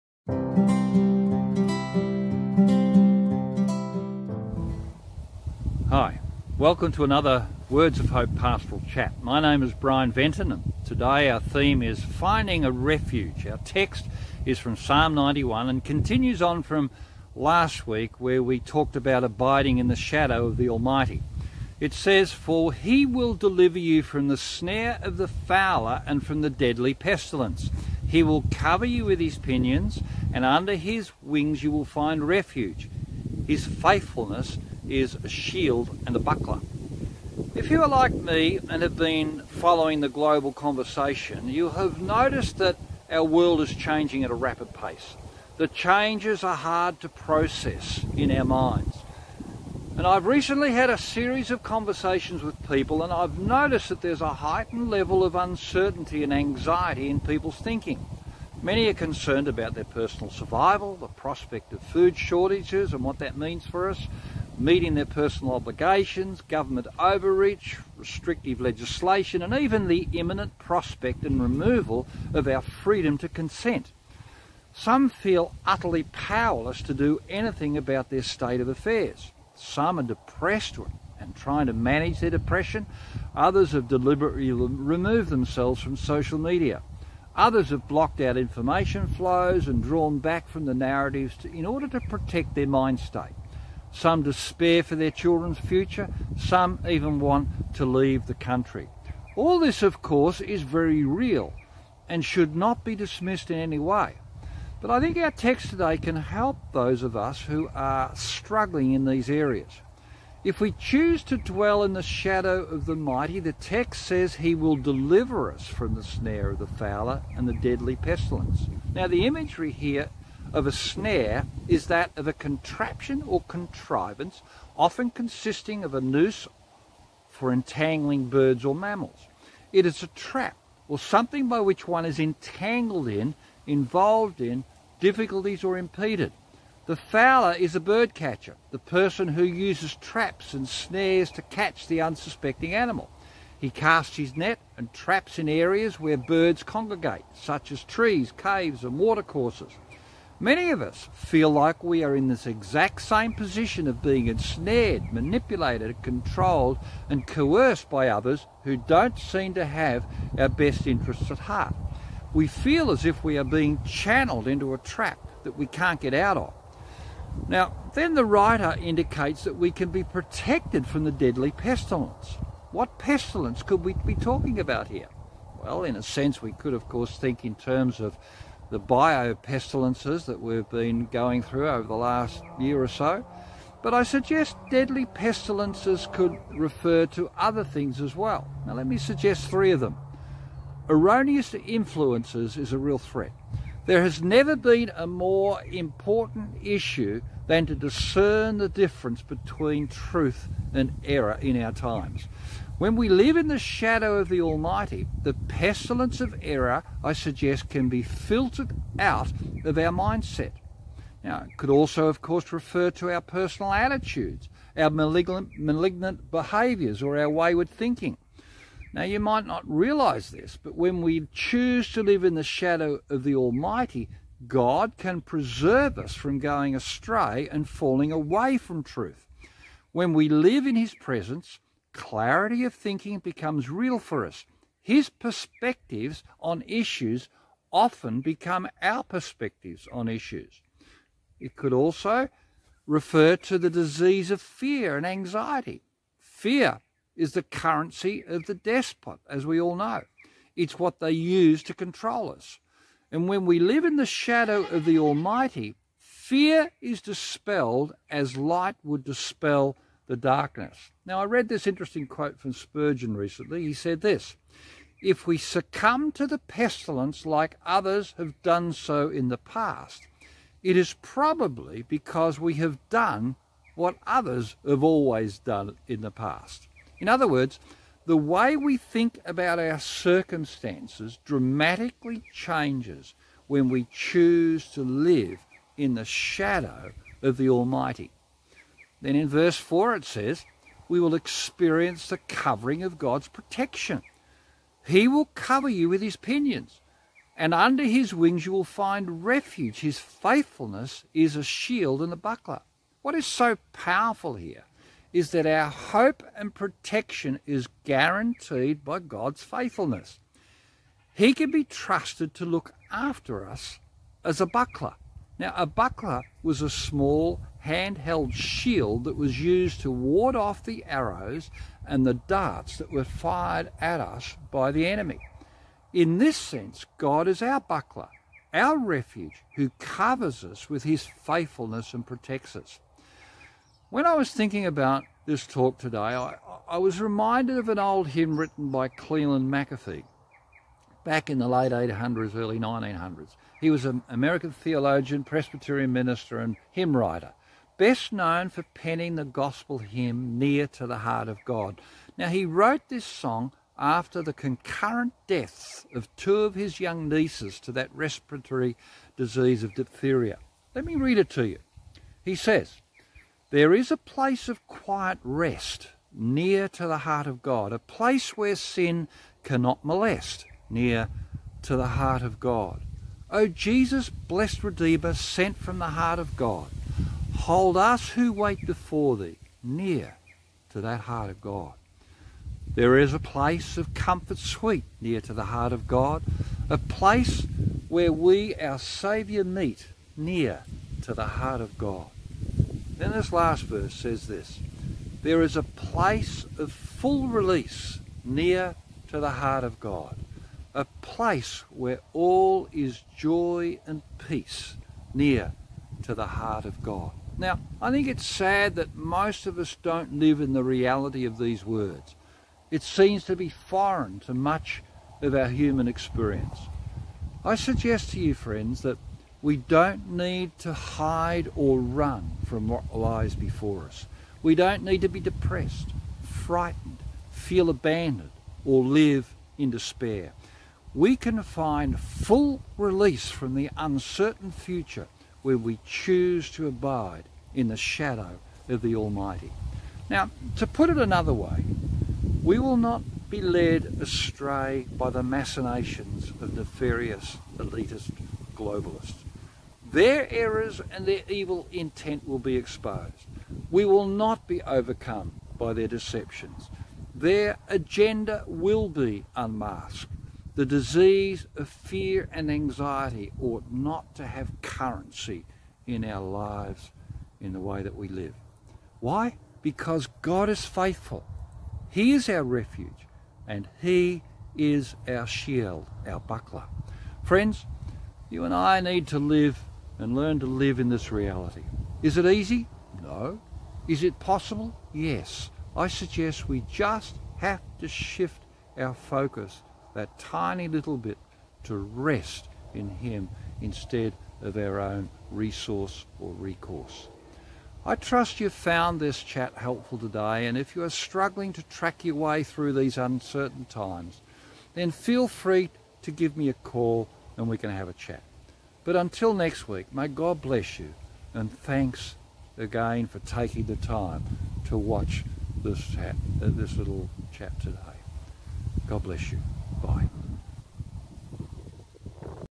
Finding A Refuge
Many people are finding it hard to process all that is happening in our global circumstance. Because change is avalanching upon us at such a rapid rate, it is creating significant uncertainty in peoples minds and getting our head around what lays before us, is not easy. This pastoral chat from Psalm 91:3-4 helps us to focus on where we can find refuge.